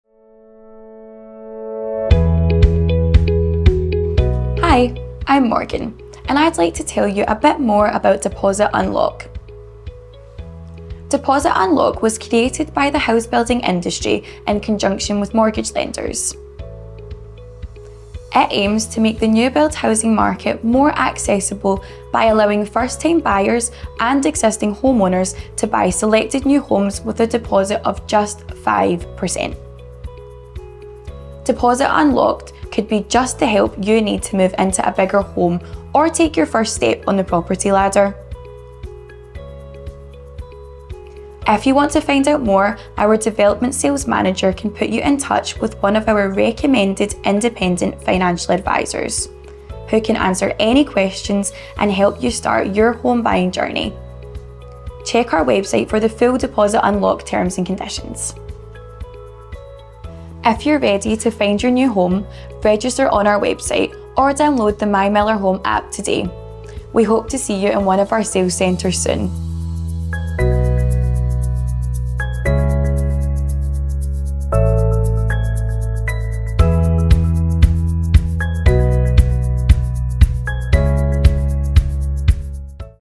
Miller Homes - Deposit Unlock - Incentive Offer
At Miller Homes, we're proud to introduce our Deposit Unlock Scheme, specifically designed to make home buying accessible and affordable for first-time buyers and existing homeowners. With just a 5% deposit, you could find yourself stepping into a brand-new home. Ready to upgrade or step onto the property ladder? Let Deposit Unlock be your guide.